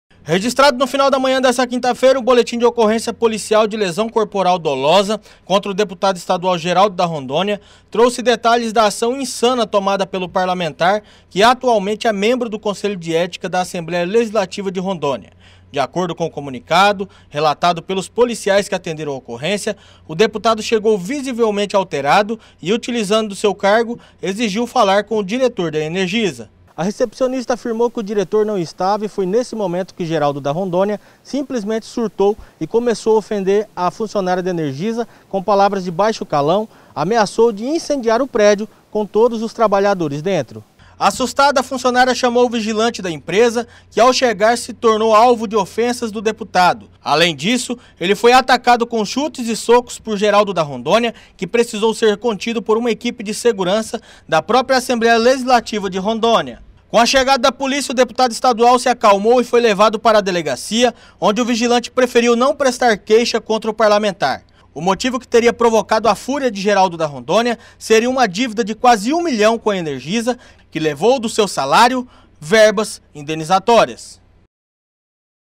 GERALDO DA RONDÔNIA
GERALDO DA RONDÔNIA AMEAÇOU MATAR FUNCIONÁRIO E ATEAR FOGO NO PRÉDIO DA ENERGISA COM TRABALHADORES DENTRO.//